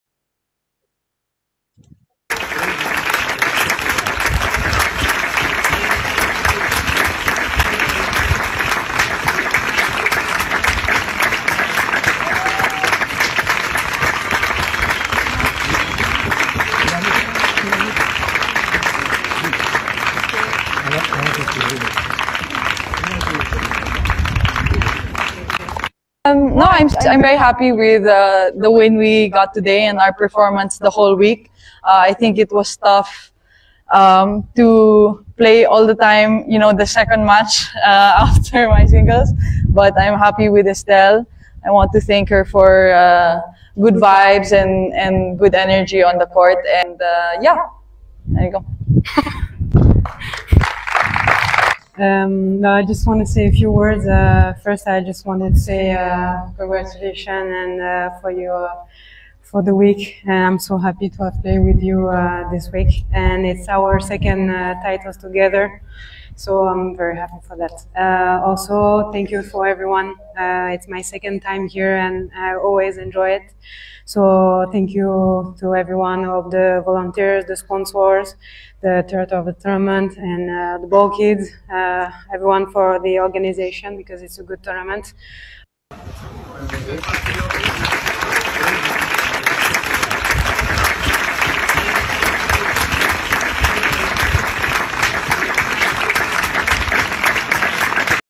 Interview with Alex Eala bags doubles crown of W100 Vitoria Gasteiz
Watch how Alex address her doubles crown in W100 Vitoria, Spain.